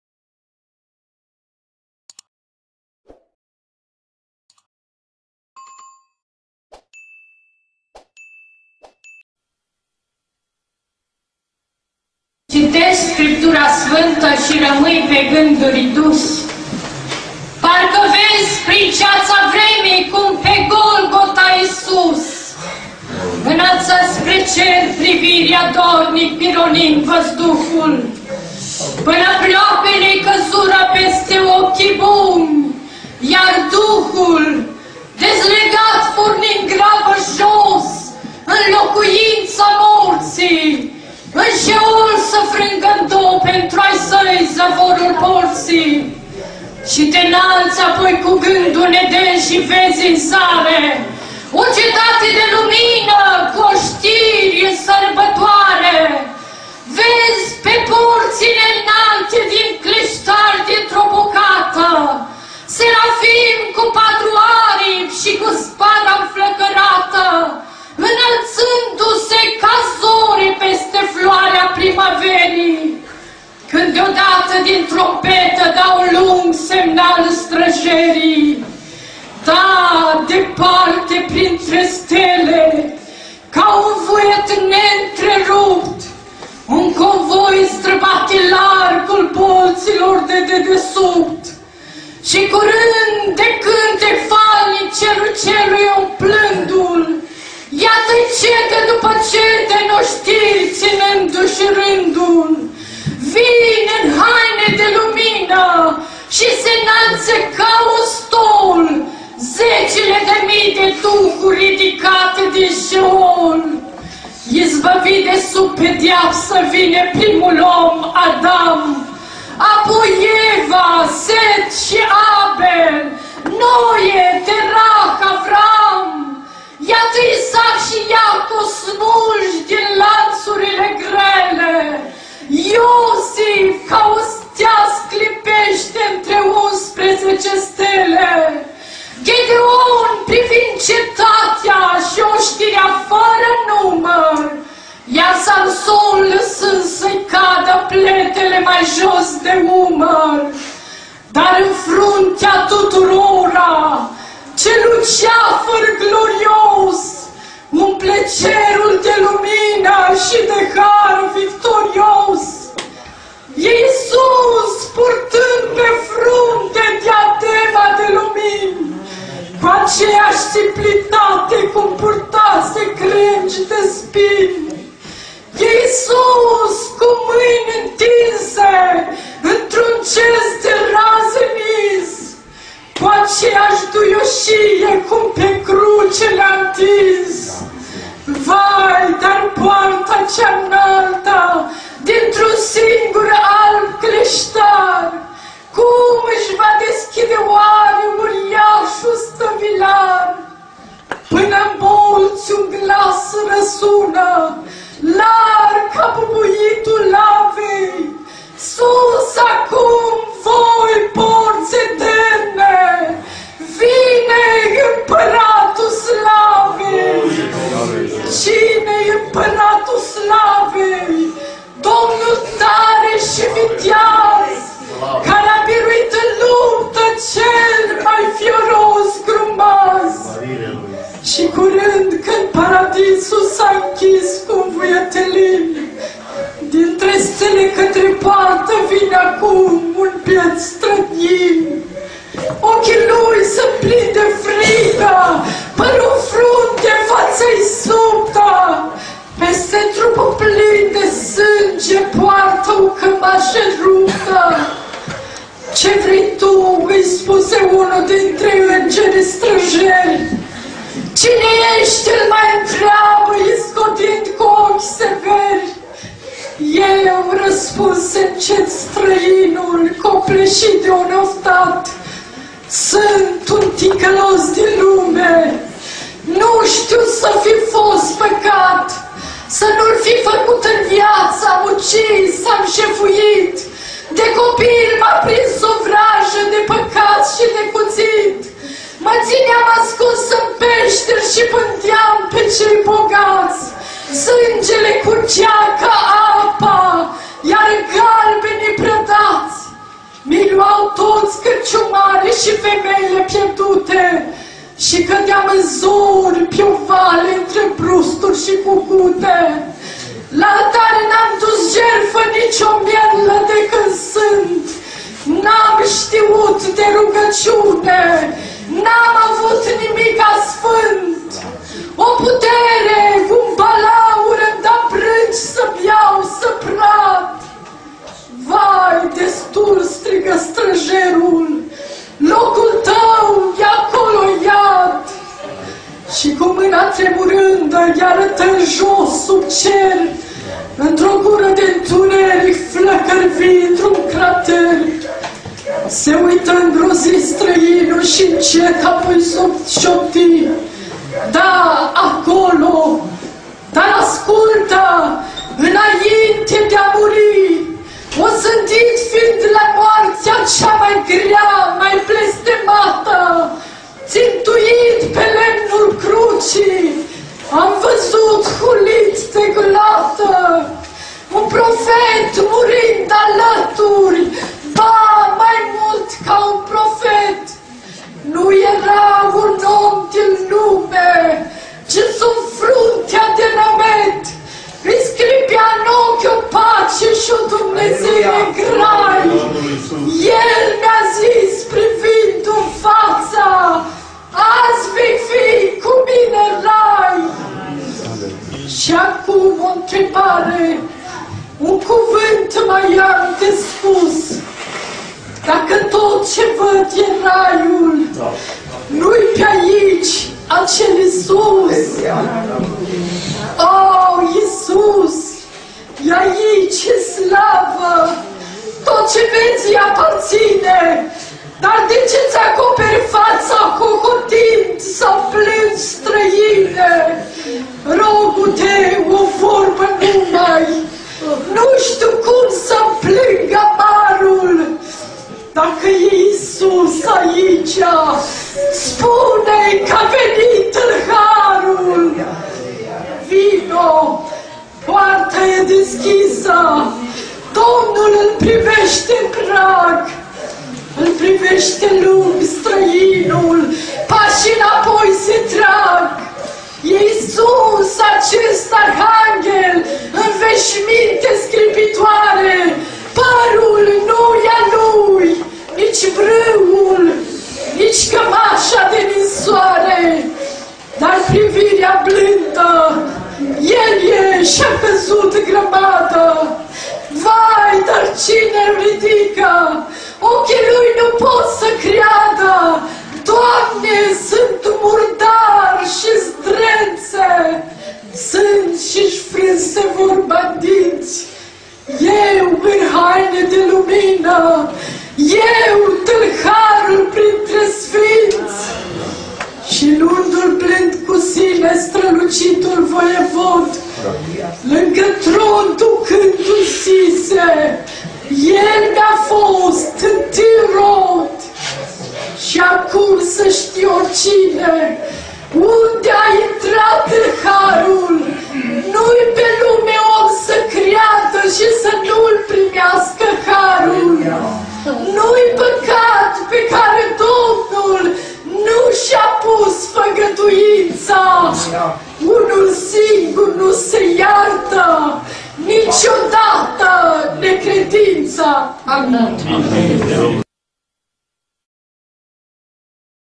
Tâlharul - Poezie - Sr. Verginica
Tâlharul
Autor: Costache Ioanid

Când citeşti Scriptura Sfântă şi rămâi pe gânduri dus,
parcă vezi prin ceaţa vremii cum, pe Golgota, Isus
înălţă spre cer privirea, dornic pironind văzduhul,
până pleoapele-I căzură peste ochii buni. Iar Duhul,
dezlegat, porni în grabă jos, în închisoarea morţii,
în Şeol, să frângă-n două, pentru-ai Săi, zăvorul porţii.

Şi te-nalţi apoi ca gândul în Eden. Şi vezi în zare
o cetate de lumină cu oştiri în sărbătoare.
Vezi pe porţile înalte de cleştar dintr-o bucată
heruvimi cu patru aripi şi cu spada-nflăcărată,
înălţându-se ca zorii peste floarea primăverii.
Dar deodată, din trompete dau un lung semnal străjerii.
Da... Departe, printre stele, ca un fulger ne´ntrerupt,
un convoi străbate largul bolţilor de dedesubt.
Şi curând, de cântec falnic cerul cerului umplându-l,
iată-i... cete după cete, ca-n oştiri ţinându-şi rândul,
vin în haine de lumină şi se-nalţă ca un stol
zecile de mii de duhuri ridicate din Şeol.

Izbăvit de sub pedeapsă, vine primul om, Adam;
apoi Eva, Set şi Abel, Noe, Terah, Avraam...
Iată... Isaac şi Iacov, smulşi din lanţurile grele,
Iosif ca o stea sclipeşte între unsprezece stele,
Moise uşurat de greul lespezilor din Horeb,
— Iosua îi stă la dreapta, iar la stânga e Caleb —;
Ghedeon privind cetatea şi oştirea fără număr,
Şi Samson, lăsând să-i cadă pletele mai jos de umăr;
David, fericit să uite de păcatele-i iertate,
Solomon şi toţi urmaşii casei binecuvântate,
toţi profeţii şi martirii ce-au murit frângându-şi dorul,
de la nepătatul Abel la Ioan Botezătorul.

Iar în faţa tuturora... ce luceafăr glorios
umple cerul de lumină şi de Har victorios?
E Isus!... purtând pe frunte diadema de lumini
cu aceeaşi simplitate cum purtase crengi de spini.
E Isus!... cu mâini întinse într-un gest de raze nins,
cu aceeaşi duioşie cum pe cruce le-a întins.
Vai!... dar poarta cea înaltă dintr-un singur alb cleştar
cum îşi va deschide oare uriaşul stăvilar?
Până-n bolţi un glas răsună larg ca bubuitul lavei:
„Sus acum, voi porţi eterne! Iată Împăratul slavei!“
„Cine-i Împăratul slavei?“ „Domnul tare şi viteaz,
care-a biruit în luptă cel mai fioros grumaz!“

Şi târziu, când Paradisul s-a închis c-un vuiet lin,
printre stele, către poartă, vine-ncet un biet străin.
Ochii lui sunt plini de frică. Păru-n frunte. Faţa suptă.
Peste trupul plin de sânge, poartă o cămaşă ruptă.
- Ce vrei tu? îi spune unul dintre îngerii străjeri.
- Cine eşti? îl mai întreabă iscodind cu ochi severi.
- Eu?... răspunde-ncet străinul copleşit de un oftat,
sunt un păcătos din lume... Nu ştiu să fi fost păcat
să nu-l fi făcut în viaţă. Am ucis. Am jefuit.
De copil m-a prins o vrajă de pahar şi de cuţit.
Mă ţineam ascuns în peşteri şi pândeam pe cei bogaţi.
Sângele curgea ca apa. Apoi galbenii prădaţi,
mi-i luau toţi cârciumarii şi femeile pierdute.
Şi cădeam în zori pe-o vale între brusturi şi cucute.
La altare n-am dus jertfă nici o mierlă de când sunt.
N-am ştiut de rugăciune. N-am avut nimica sfânt.
O putere, un balaur, îmi da brânci să beau, să prad...
- Vai, destul... vorbi străjerul. Locul tău e-acolo-n Iad.
Şi, cu mâna lui întinsă, i-arătă în jos sub cer,
într-o gură de-ntuneric, flăcări vii într-un crater.
Se uită-ngrozit străinul. Iar apoi încet şopti:
- Da... acolo... Dar ascultă. Înainte de-a muri,
osândit fiind la moartea cea mai grea, mai blestemată,
ţintuit pe lemnul crucii, am văzut, hulit de gloată,
un profet murind alături. Ba... mai mult ca un profet!
Nu era un om din lume. Ci, sub fruntea de nămet,
Îi plutea în ochi o pace... şi-o dumnezeire-n grai.
El mi-a spus privindu-mi faţa: Azi vei fi cu Mine-n Rai!
Şi acum o întrebare... un cuvânt mai am de spus:
Dacă tot ce văd e Raiul, nu-i pe-aici acel Isus?...
- O! Isus? E-aici în slavă... tot ce vezi Îi aparţine.
Dar de ce-ţi acoperi faţa hohotind? Sau plângi, străine?
- Rogu-te, o vorbă numai... Nu ştiu cum să-mi spun amarul...
Dacă e Isus aicea... spune-I c-a venit tâlharul.
- Vino!... Poarta e deschisă... Domnul îi vorbeşte-n prag.
Îl priveşte lung tâlharul. Paşii înapoi se trag...
E Isus acest arhanghel în veşminte sclipitoare?
Părul... Nu-i al Lui. Nici brâul. Nici cămaşa de ninsoare.
Dar... privirea blândă... Ochii. El e! Şi-a căzut grămadă.
Cum? Dar cine îl ridică? Ochii lui nu pot să creadă.
- Doamne, sunt murdar... şi-n zdrenţe. Sunt...
şi-şi frânse vorba-n dinţi...
- Eu... în haine de lumină? Eu, tâlharul, printre sfinţi?
Şi luându-l blând cu Sine, strălucitul Voievod,
lângă tron ducându-l, zise: „El Mi-a fost întâiul rod!“
Şi acum să ştie oricine: Unde a intrat tâlharul,
nu-i pe lume om să creadă şi să nu-l primească Harul!
Nu-i păcat pe care Domnul nu şi-a pus făgăduinţa.
Unul singur nu se iartă niciodată: Necredinţa!